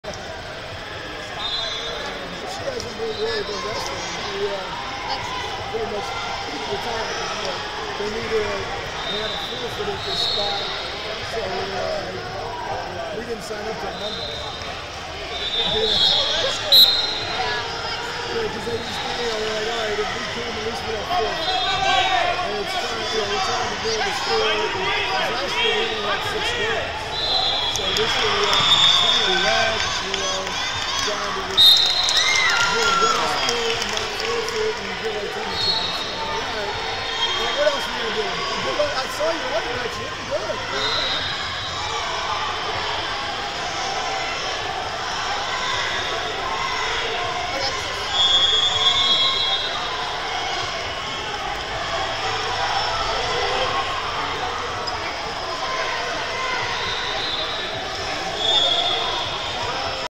105 Lbs Round 4 (6 Team) - Alexis Lauta, Pennsylvania Vs Madelyn Little, Virginia 2098
Flosports-partner-access/2023 Junior National Duals And 14U Girls Duals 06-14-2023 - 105 Lbs Round 4 (6 Team) - Alexis Lauta, Pennsylvania Vs Madelyn Little, Virginia 2098